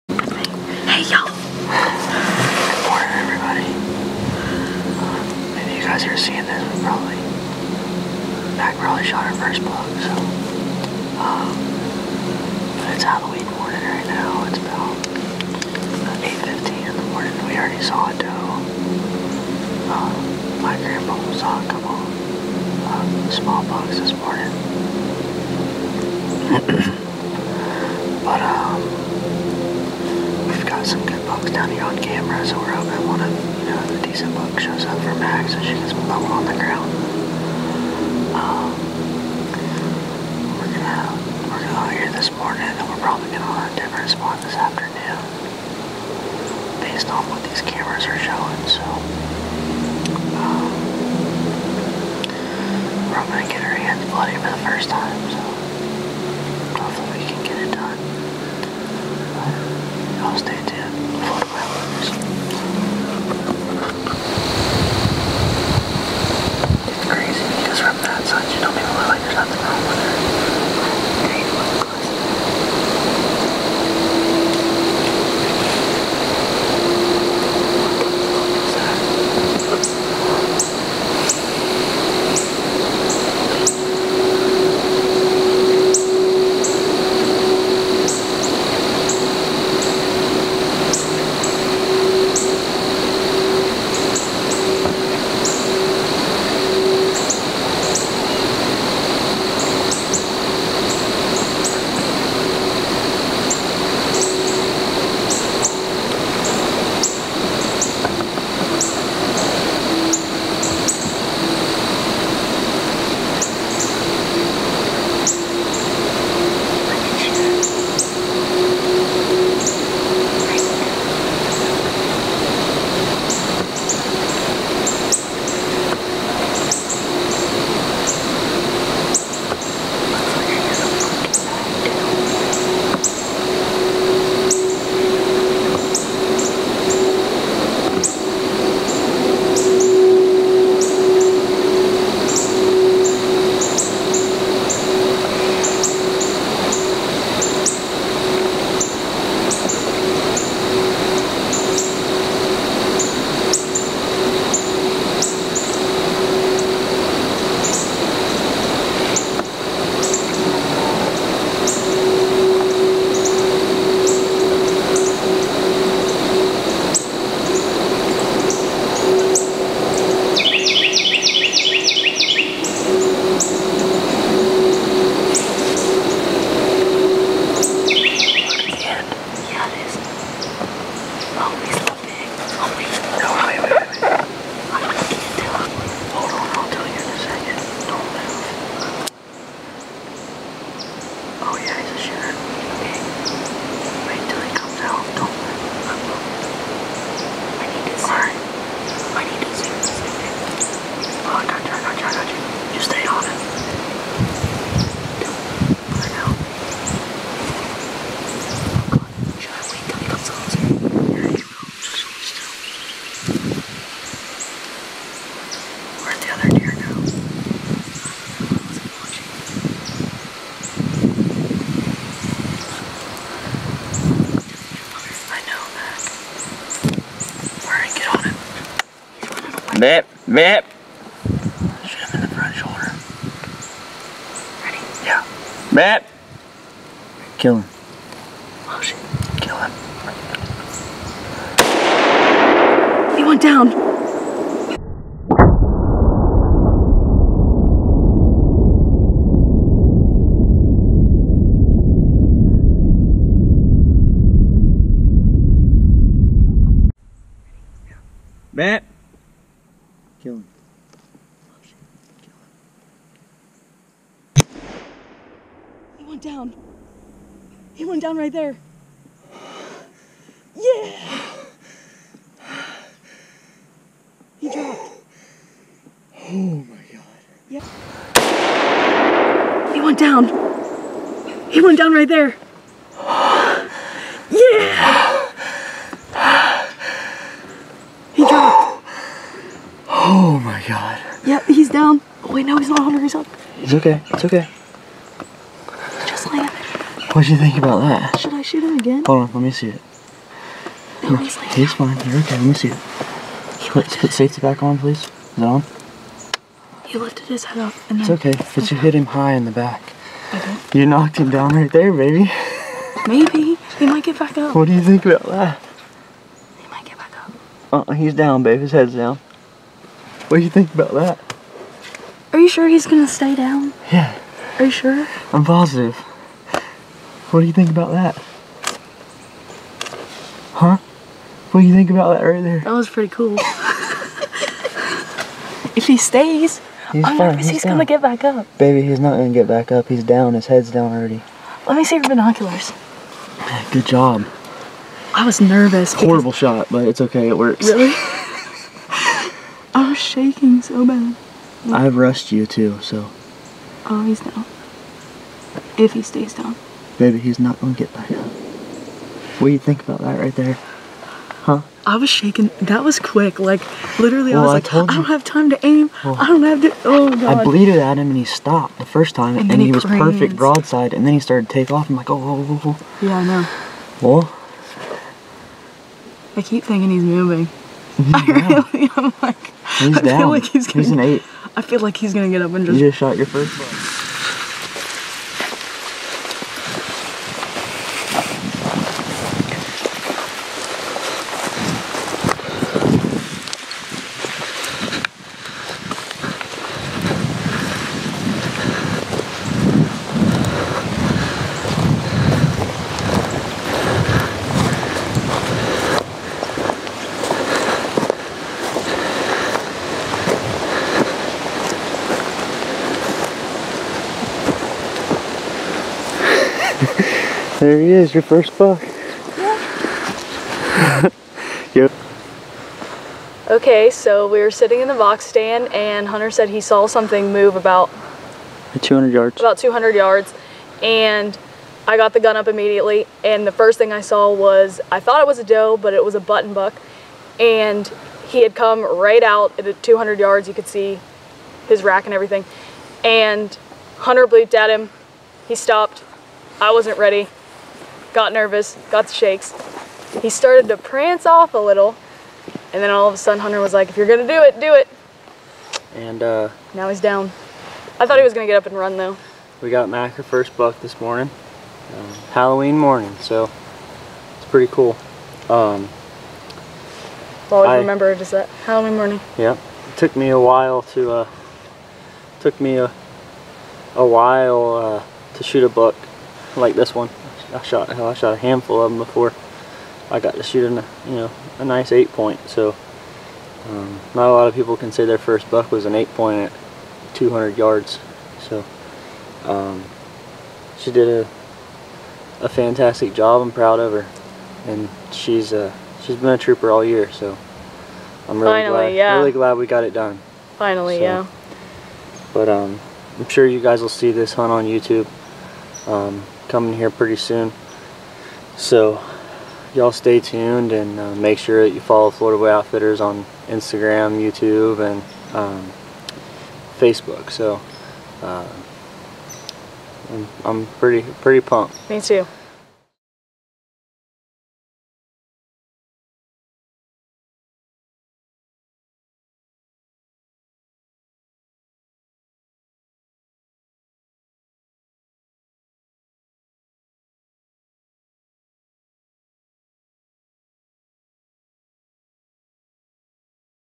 MACK SHOOTS HER FIRST BUCK! 150+ Yard shot!
Hello everyone, welcome back to the @floridaboyoutfitters YT channel! Today's video is taking place in south GA! This is my girlfriend Mack's first deer/buck! She made a great shot at about 165 yards! Please check it out, if you enjoyed the video please like, sub and leave a positive comment congratulating her! Very special S/O to the following companies
-TREEZYN CAMO
-RACKGETTER SCENTS AND LURES
-SPYPOINT TRAIL CAMERAS